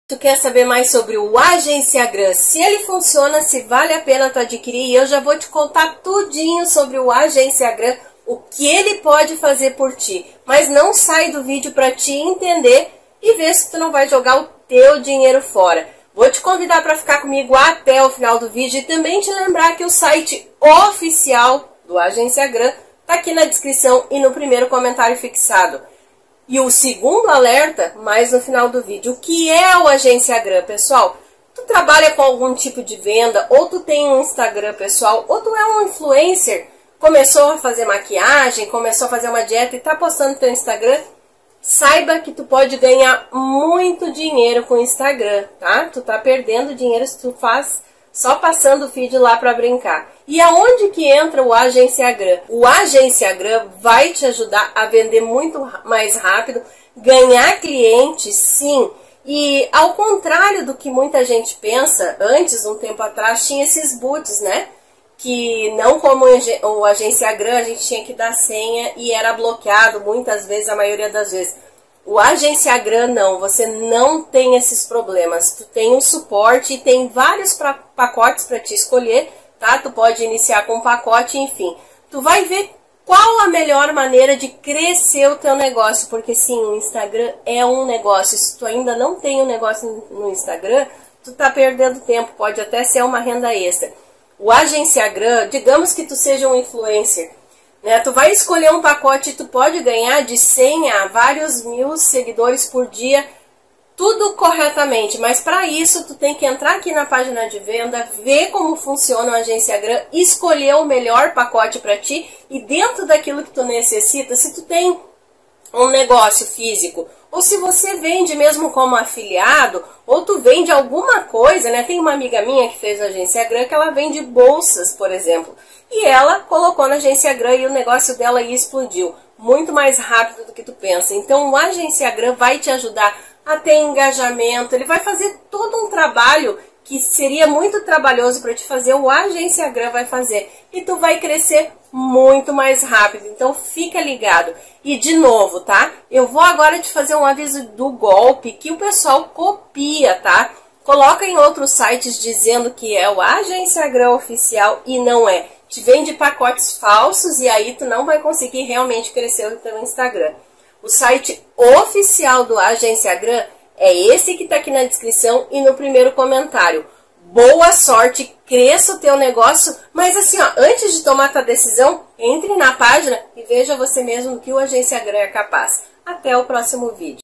AGENCIAGRAM Funciona? ((ALERTA!)) Agenciagram Vale a Pena para Seguidores? AGENCIAGRAM é Confiável?
✅Nesse video eu explico exatamente pra você tomar cuidado e como funciona o Agenciagram e passo também um  super alerta pra você não está caindo em nenhum golpe.

 Se você quer aprender a como crescer seu instagram com o Agenciagram, esse video é ideal para você.

✅O que é o Agenciagram?
Então, vamos lá, o Agenciagram é uma plataforma de troca de seguidores, onde pessoas comuns usam essa plataforma para seguir pessoas em troca de seguidores. O grande desse segredo que faz o Agenciagram funcionar é que o seu perfil dentro dessa plataforma é visto como perfil  recomendado e assim as pessoas irão seguir o seu perfil de forma orgânica,  rescendo o número de seguidores que você.

✅Agenciagram são seguidores reais?
Sim, com o Agenciagram você garante diversos seguidores brasileiros e reais te seguindo todos os dias.

✅Agenciagram Funciona?
Sim, o Agenciagram funciona e vai servir muito bem pra você, se você quer crescer seu instagram, ele é muito bom, pois você terá todos os dias novo seguidores chegando em seu instagram.

✅Agenciagramé Confiável ?
Sim, o Agenciagram é totalmente confiável e super bem avaliado pelas pesssoas que usam ele hoje em dia.

✅O Agenciagram é bom mesmo?
Sim, o Agenciagram é uma das melhores ferramentas do mercado nessa área, no site oficial contém diversos depoimentos de pessoas que comprara me obtiveram otimos resultados e para você poder tirar qualquer dúvida e caso tenha alguma pode botar no comentário aqui do video.

✅Onde comprar o Agenciagram?
Somente pelo site oficial o mesmo que deixei aqui na descrição e tome muito cuidado ao comprar o por outro site para não acabar perdendo o seu investimento.

Se você esta buscando uma oportunidade de ganhar mais seguidores no instagram o Agenciagram é ideal pra você!!

00:05 - Agenciagram
00:30 - Agenciagram é Bom
01:30 - Agenciagram é Confiável
02:20 - Agenciagram Funciona
02:43 - Agenciagram Seguidores